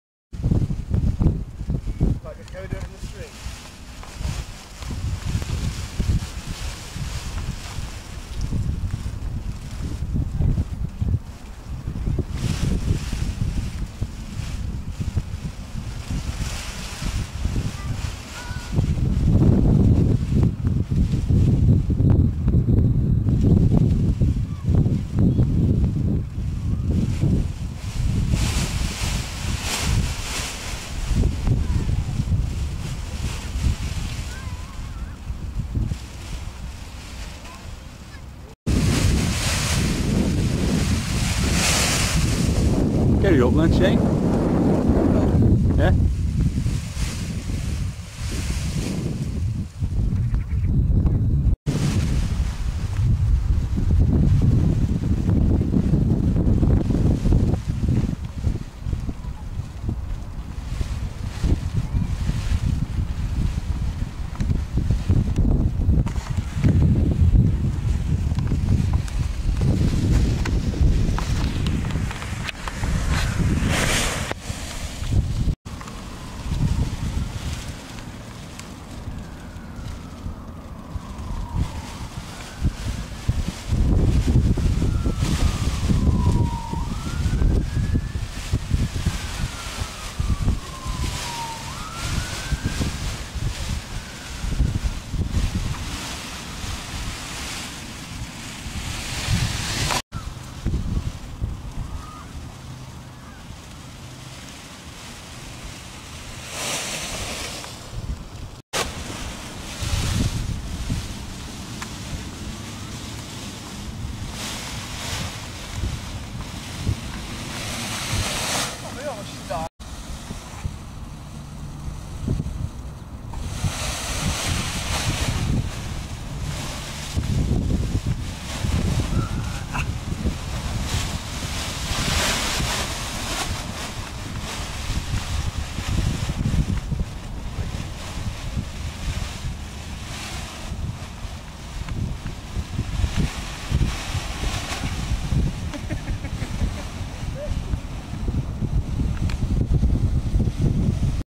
Paragliding ground handling
Trying to do some ground handling but the wind decided to die off as soon as i unpacked the glider.